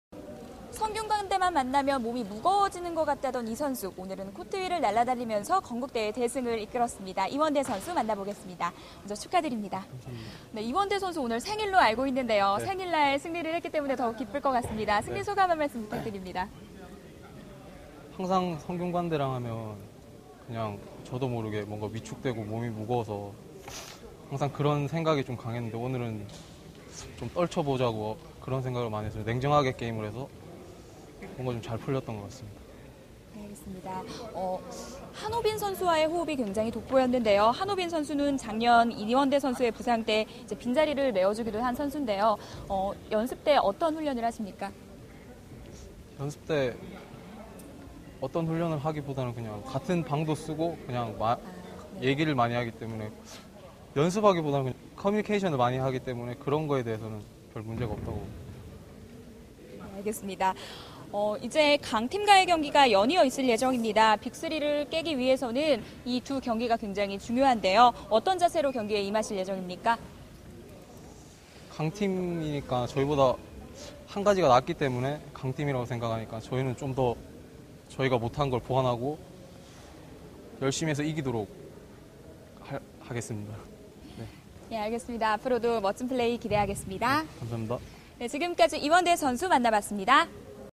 [대학리그] 건국대 이원대 인터뷰
건국대는 27일 건국대 충주캠퍼스 체육관에서 열린 2011 KB국민은행 대학농구리그에서 결정적인 순간 3점슛 4개를 적중시키며 상대의 추격의지를 꺾은 이원대의 활약에 힘입어 성균관대를 95-81로 대파했다. 이날 생일을 맞은 이원대에게 승리소감과 더불어 강팀과의 경기를 어떻게 대비할 것인지에 대해 언급하고 있다.